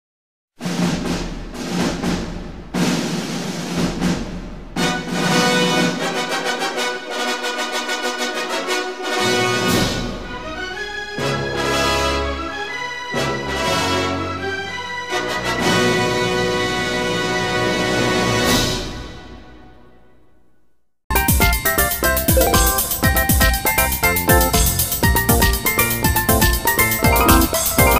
20th Century Fox/Fox Studios Bahamas (1996, prototype)
If Fox Studios Bahamas released "Girl 6 (1996)" instead of Fox Searchlight Pictures